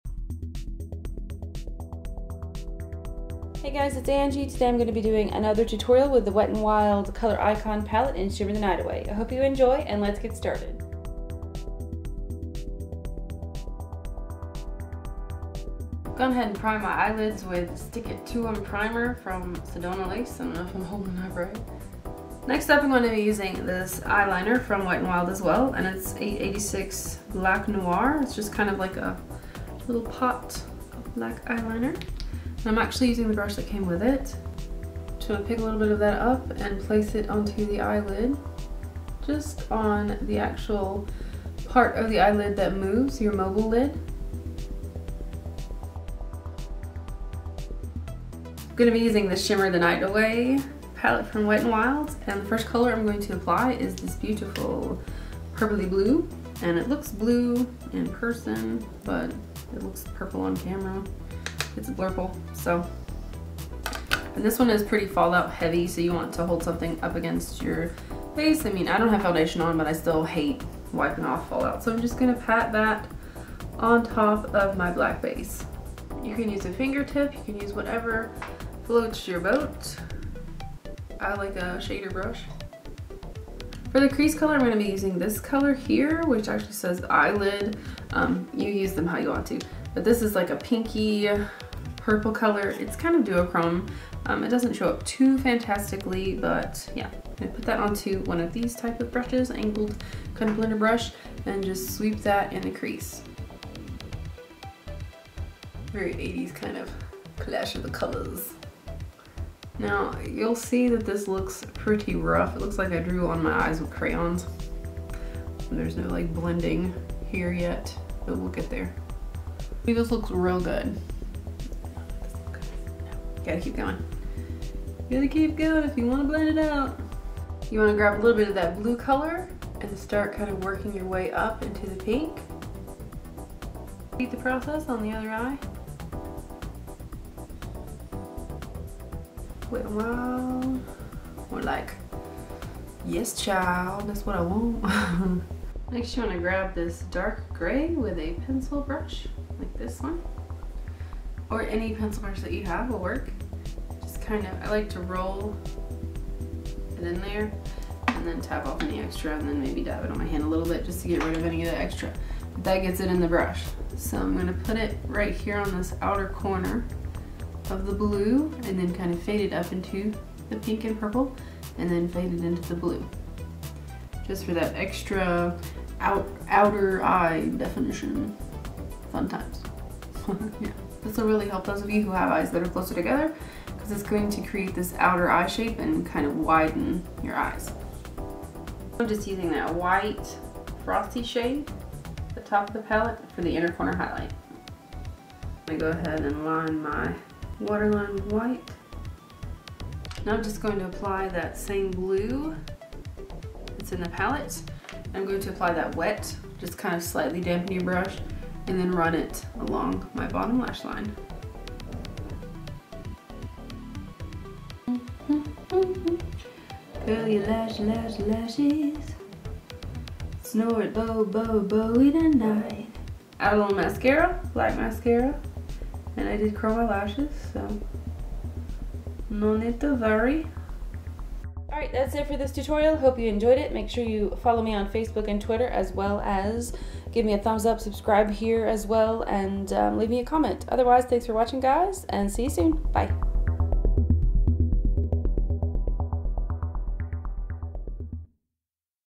MIDNIGHT BLUE: With the Shimmer All Night Palette
Hey guys! Here's a another quick eye tutorial using the Wet N Wild Shimmer the Night Away palette.
xx
Angie

PRODUCTS:
EYES-
Wet N Wild Color Icon Palettes in Shimmer the Night Away
Sedona Lace Eye Primer Stick it To 'Em*
Clinique High Impact Mascara in 01 Black
Rimmel Kohl Eyeliner in White
FACE-
Makeup Forever HD Foundation in 115
Rimmel Stay Matte Powder
Tarte Amazonian Clay Blush in Dollface
LIPS-
MAC Lipglass in Please Me

FTC: All items marked * were provided by PR. All opinions are my own.